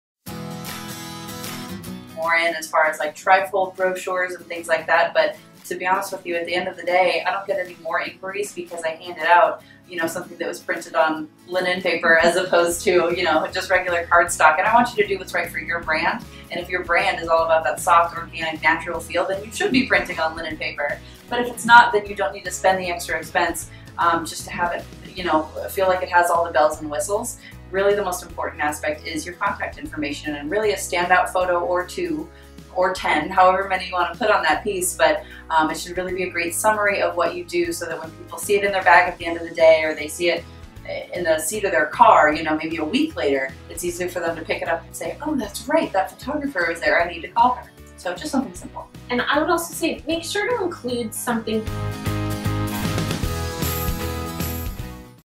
Professional Pooch - Showcase Your Business at Local Events - Snippet 04
This video is overflowing with everything you need to know about having a booth at a local event. We cover finding the right events for your business, what materials you need (and where to get them!), networking, incentives to book sessions on the spot, and so much more. PLUS, we go through our individual booths in detail and show you what some event-day conversations sound like. A bonus Event Checklist PDF is included!